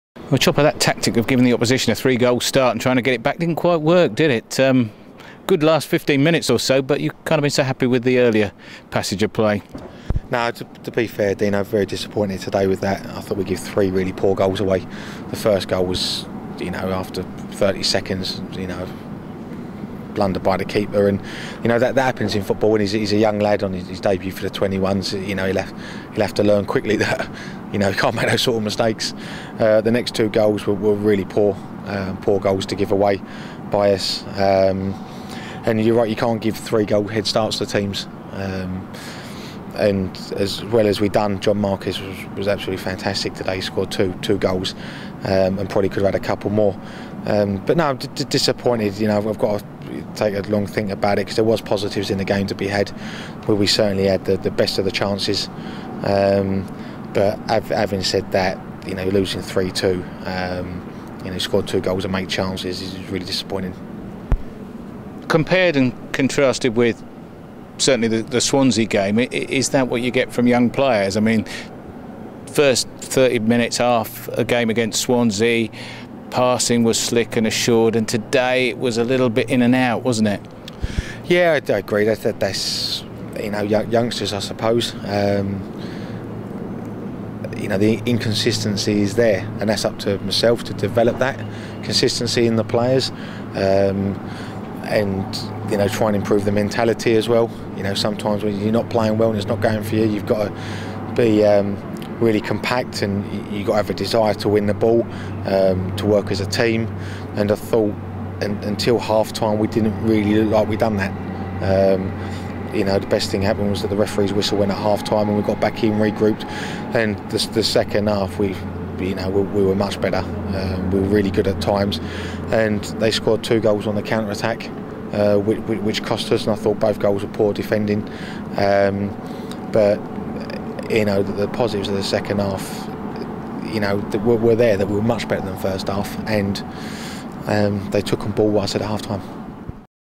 EXCLUSIVE: Neil Harris on U21s Bristol City defeat 02-09-13
Millwall U21s couldn't repeat the success of their last two games, as they were beaten 3-2 by Bristol City U21s at The Den. Look out for the full interview on Lions Player soon